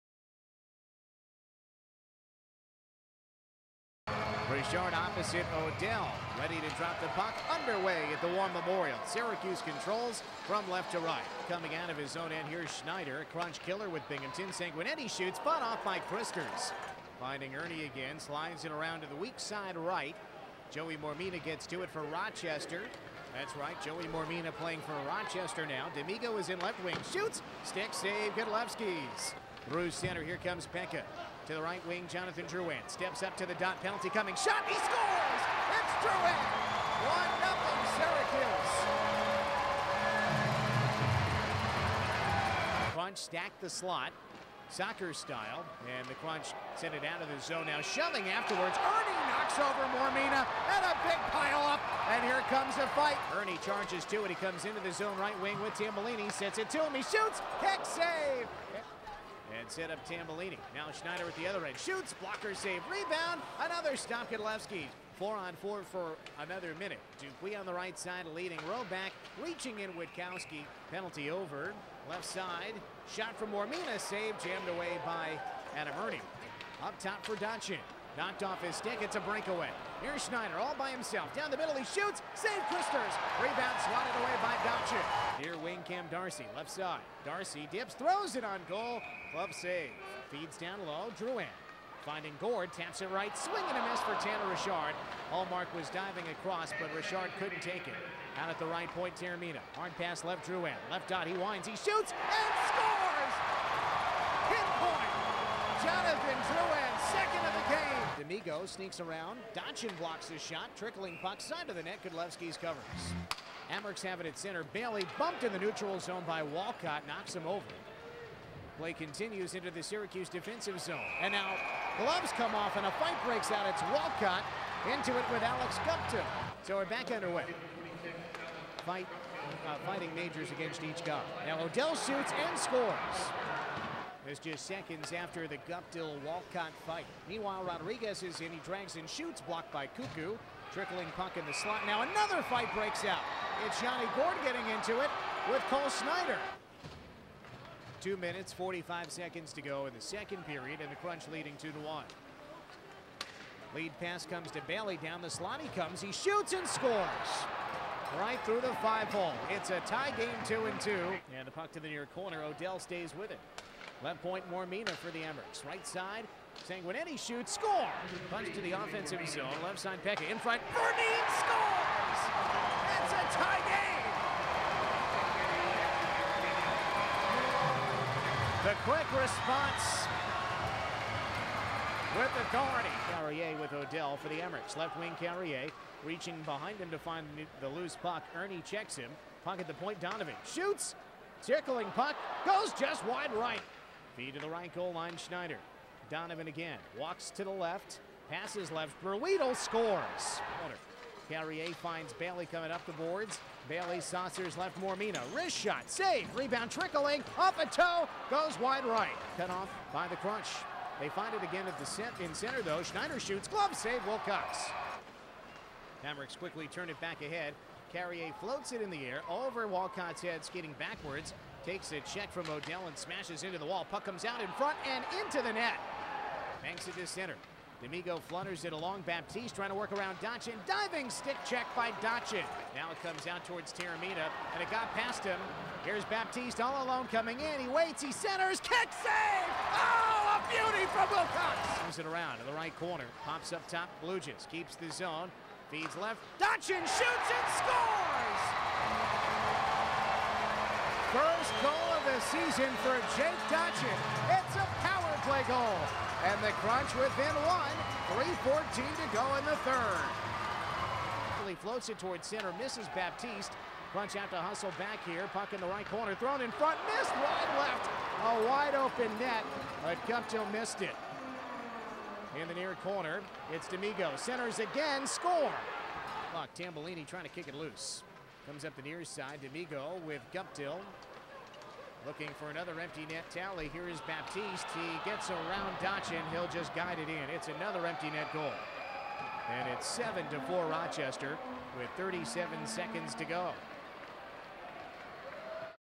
Game 61 | Syracuse vs Rochester l March 12, 2016
Syracuse Crunch​ unable to hold off rivals  Rochester Americans​ in a physical match-up after taking an early lead in the game tonight at the War Memorial Arena. Jake Dotchin gets his first goal of the season, and Johnathan Drouin nets two goals in the game.